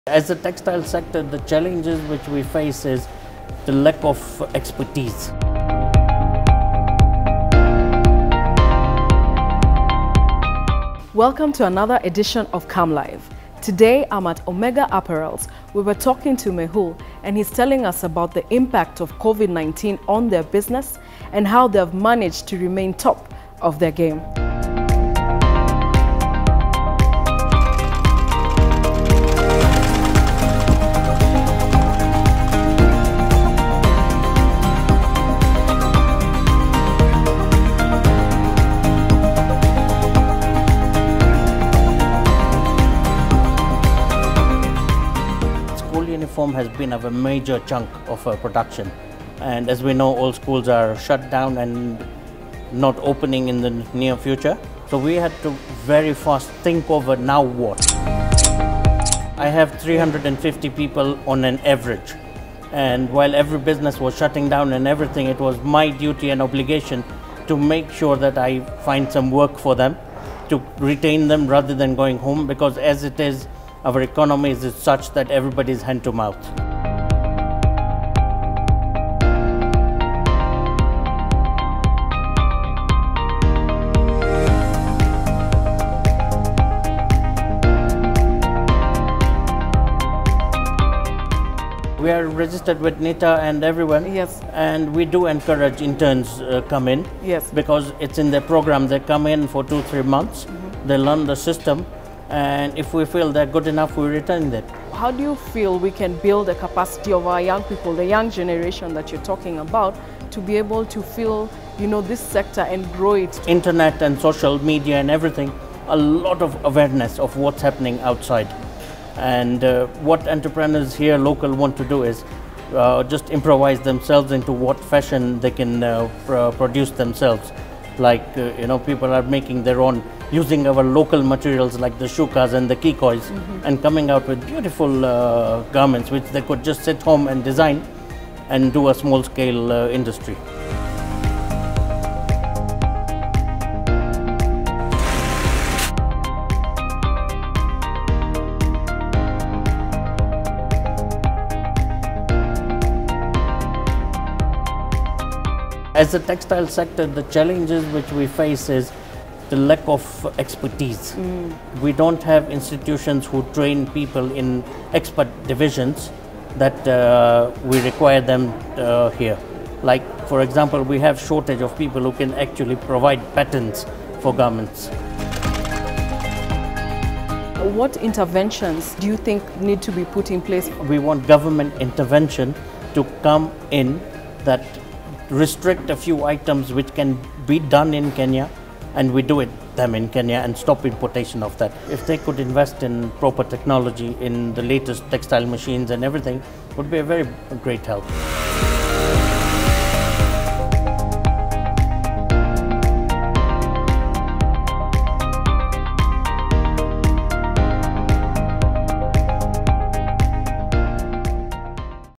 KAM Live visits Omega Apparels
Mehul Shah, Omega Apparels CEO, has a chat on KAM Live and highlights the impact of COVID 19 on his business as well as the need for skills development in the manufacturing sector.